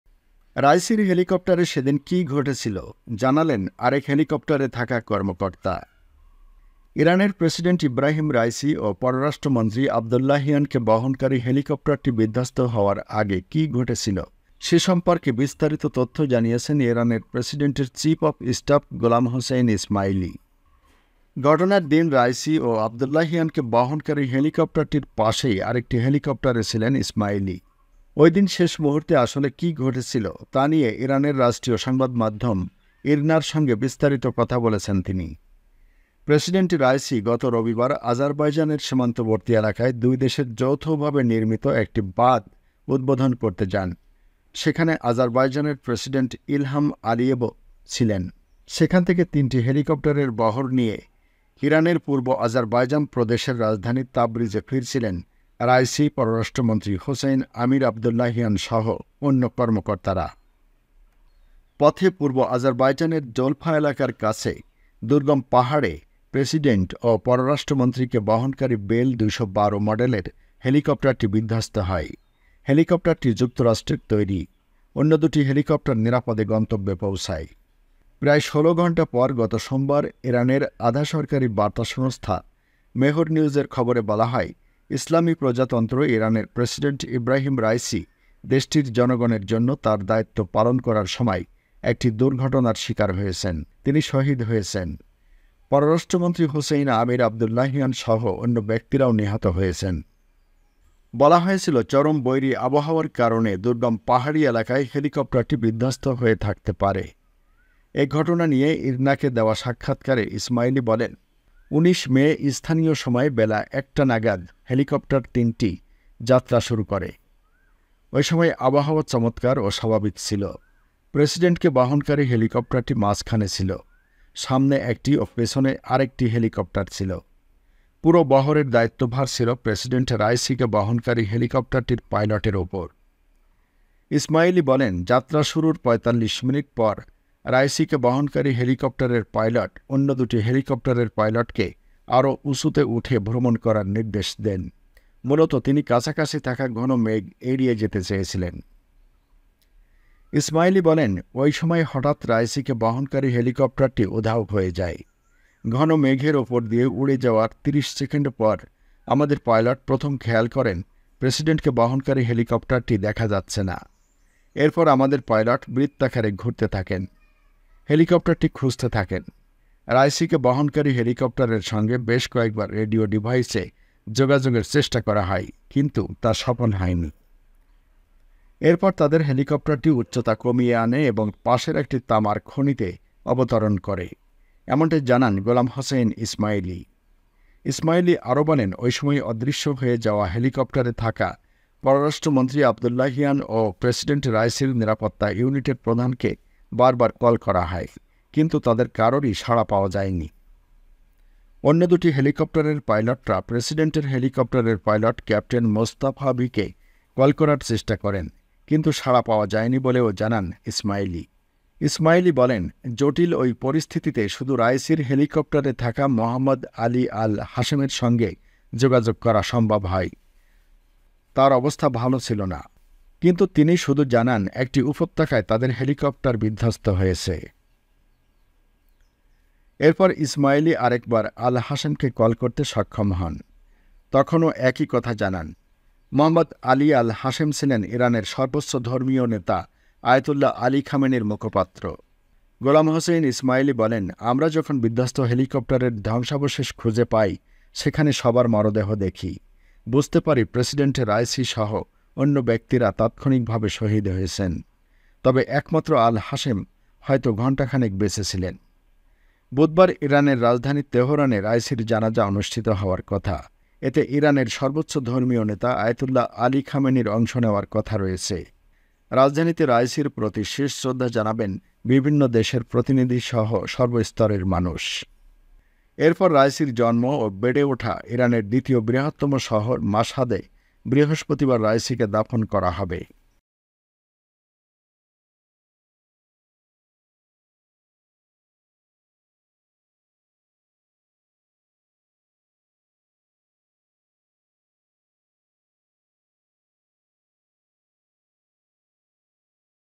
প্রেসিডেন্ট রাইসির হেলিকপ্টারে সেদিন কী ঘটেছিল সব জানা গেল Raisi died on helicopter crash
ইরানের প্রেসিডেন্ট ইব্রাহিম রাইসি ও পররাষ্ট্রমন্ত্রী আবদোল্লাহিয়ানকে বহনকারী হেলিকপ্টারটি বিধ্বস্ত হওয়ার আগে কী ঘটেছিল, সে সম্পর্কে বিস্তারিত তথ্য জানিয়েছেন ইরানের প্রেসিডেন্টের চিফ অব স্টাফ গোলাম হোসেইন ইসমাইলি।

ঘটনার দিন রাইসি ও আবদোল্লাহিয়ানকে বহনকারী হেলিকপ্টারটির পাশেই আরেকটি হেলিকপ্টারে ছিলেন ইসমাইলি। ওই দিন শেষ মুহূর্তে আসলে কী ঘটেছিল, তা নিয়ে ইরানের রাষ্ট্রীয় সংবাদমাধ্যম ইরনার সঙ্গে বিস্তারিত কথা বলেছেন তিনি।